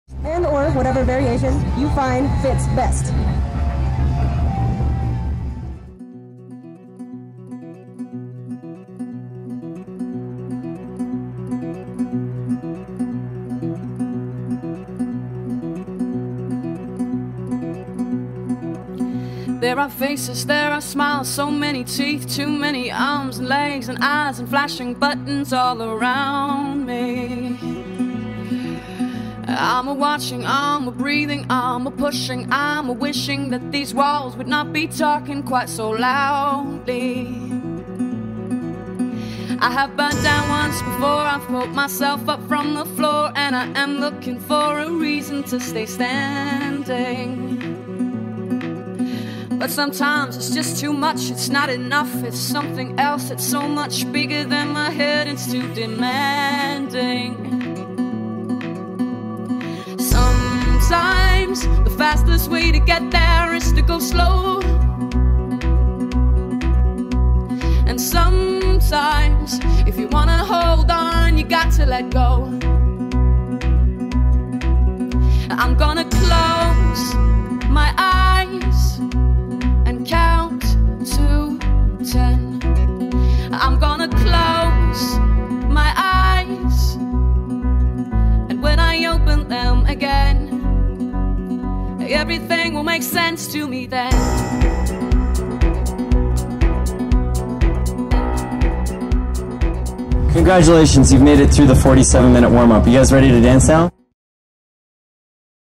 The Sacred G's 2020 Teaching Demo
Street Dance Workshop at Gem & Jam Festival, 2020.